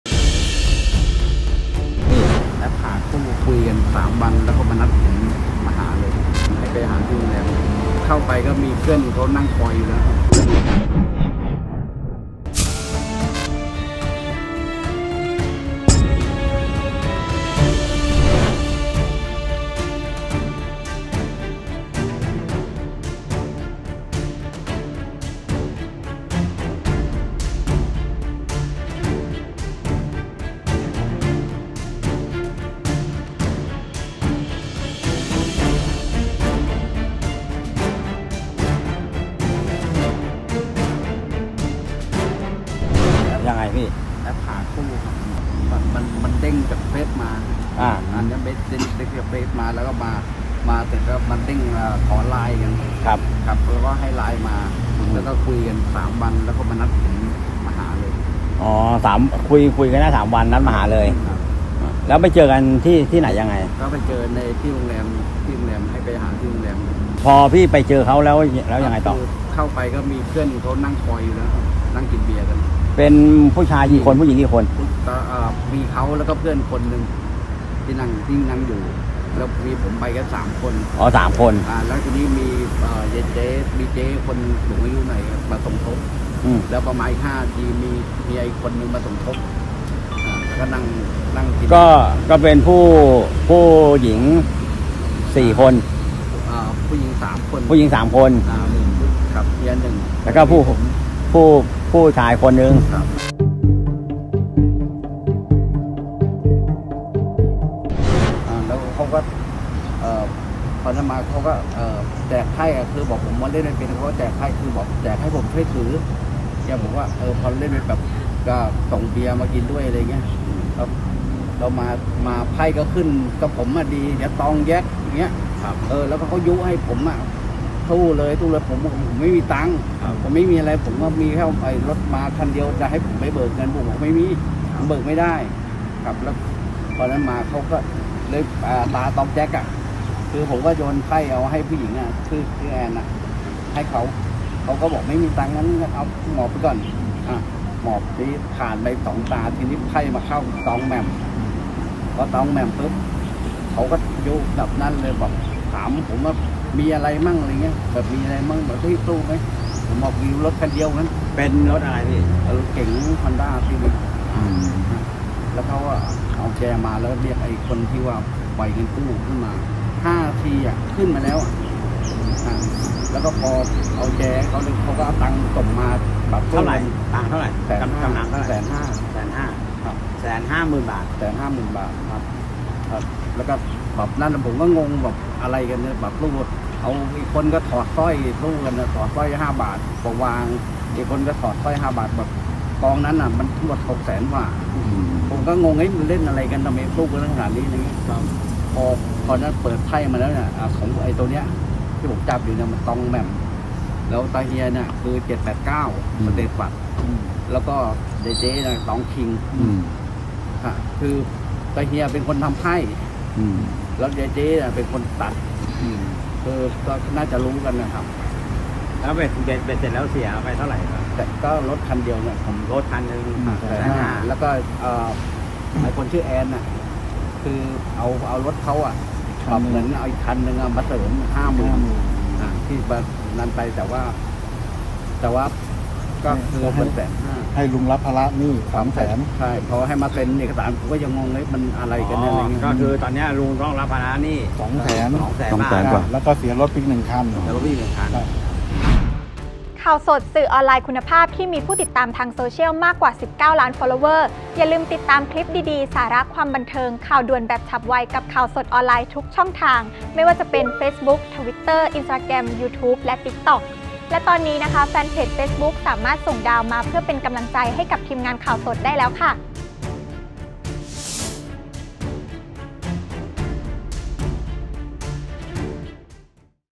ลุงสะอื้น! เล่นแอพหาคู่ 3 วัน สาวนัดเจอในโรงแรม สุดท้ายชวนเล่นไพ่ หมดเงิน 2 แสน อ้าวเธอ!: Khaosod TV
ลุงสะอื้น!! เล่นแอพหาคู่ 3 วัน สาวนัดเจอในโรงแรม สุดท้ายชวนเล่นไพ่ หมดเงิน 2 แสน อ้าวเธอ!!